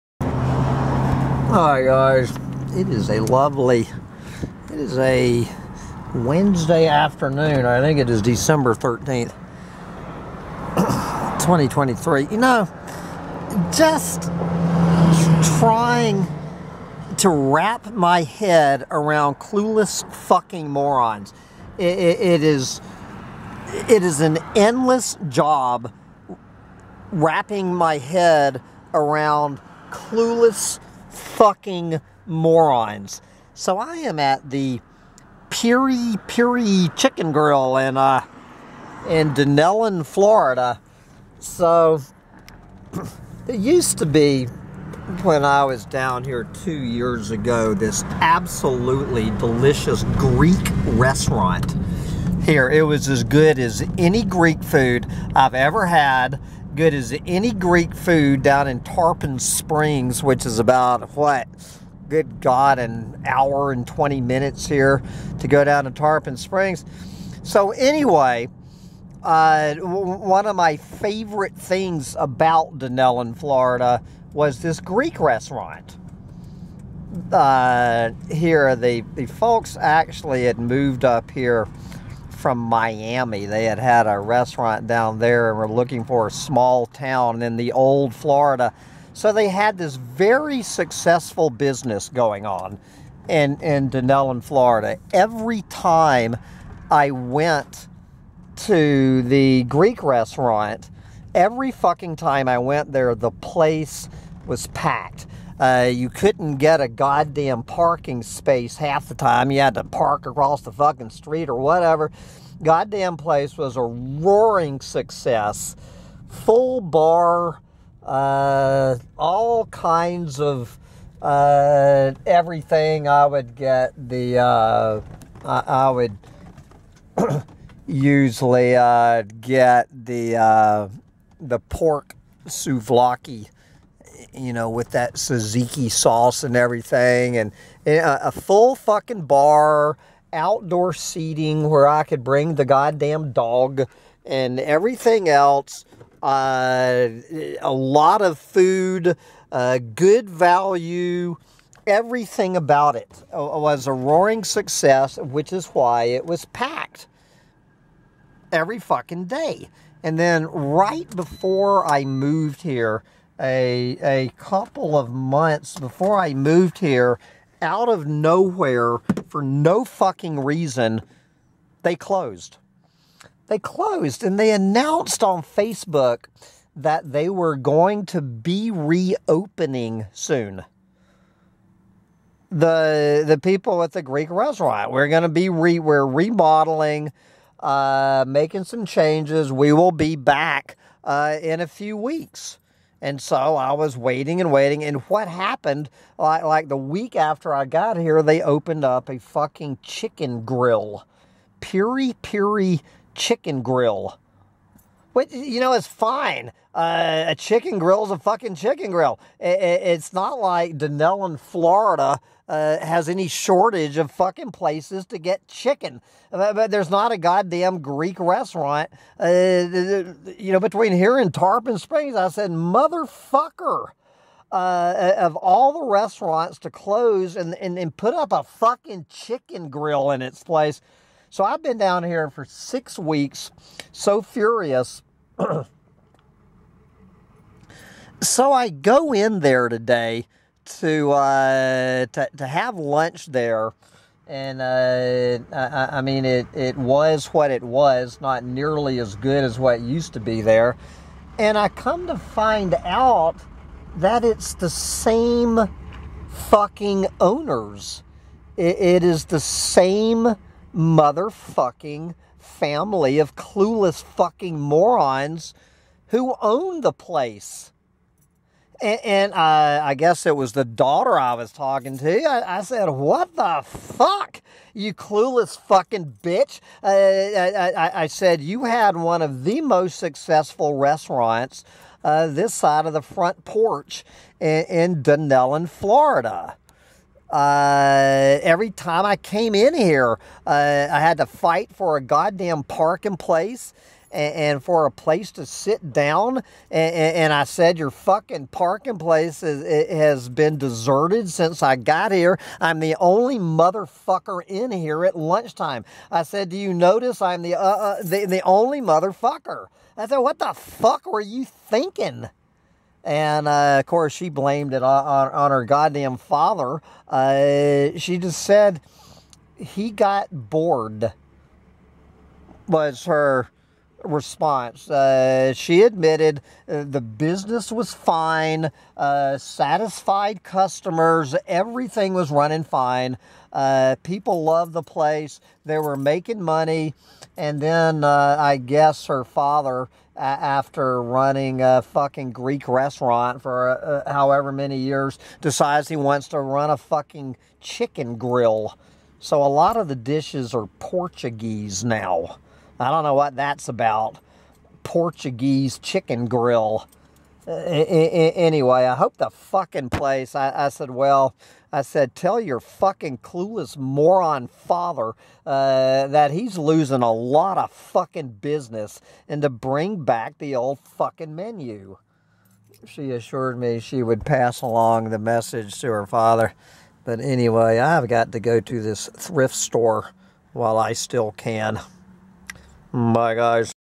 The Never-Ending Mystery of What Makes Clueless Morons Tick
Why do I want to slap about 90 percent of the folks I meet? When did we become such a melting pot of imbeciles (I had to look up the spelling of "imbecile" because five years of college was not enough)?